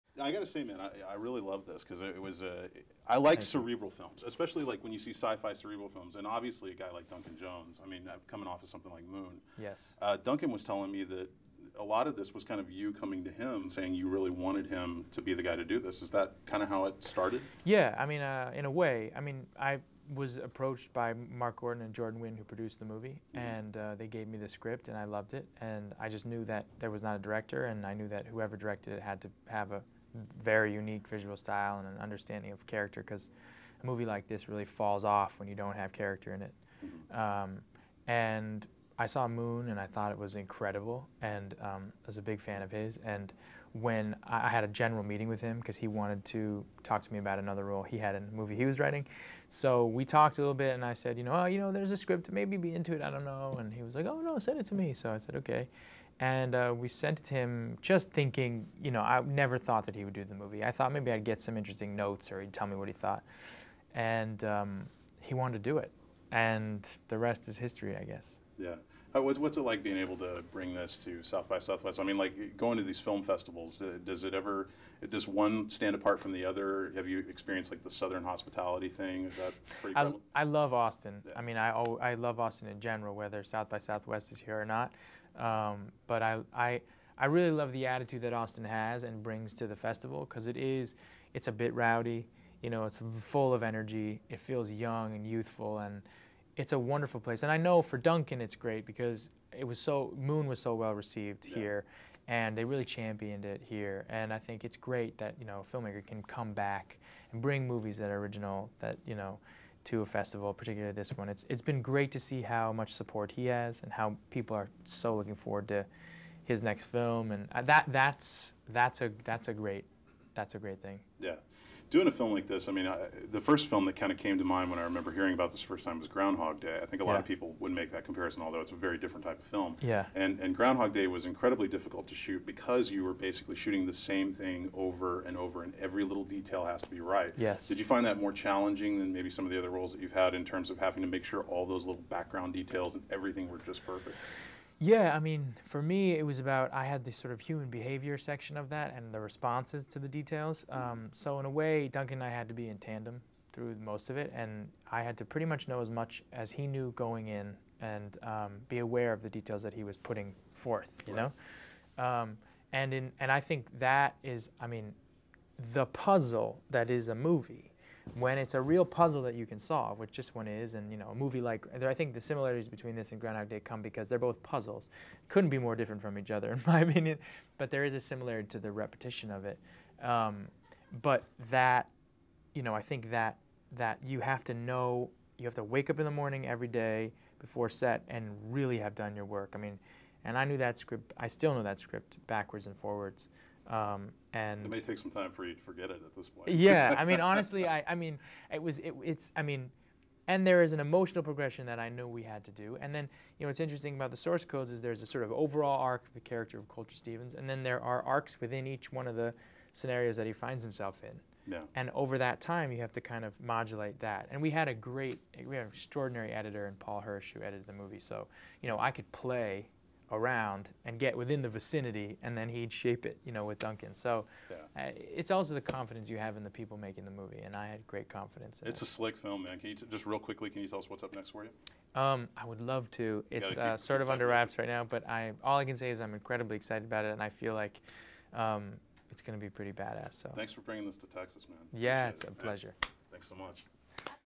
Jake Gyllenhaal SOURCE CODE interview at SXSW 2011 with Bigfanboy.com
Mark Walters of talks one-on-one to Jake Gyllenhaal, the star of director Duncan Jones' SOURCE CODE.  Jake dicsusses the beginnings of the project, the challenges of shooting the same moments over and over, and more in this laid back interview.

The video is property of Bigfanboy.com and should not be duplicated in any way without permission.